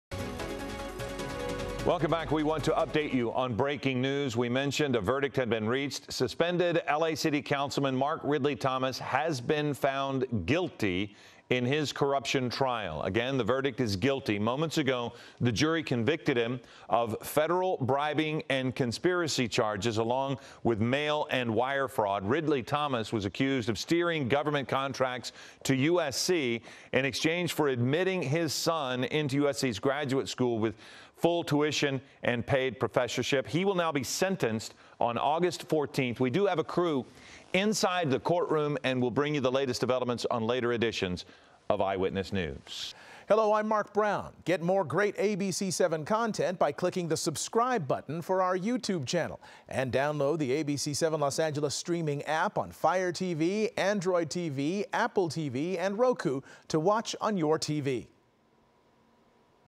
Suspended LA Councilman Mark Ridley-Thomas convicted of bribery, conspiracy in corruption trial
Suspended Los Angeles City Councilman Mark Ridley-Thomas was convicted of federal bribery and conspiracy charges. MORE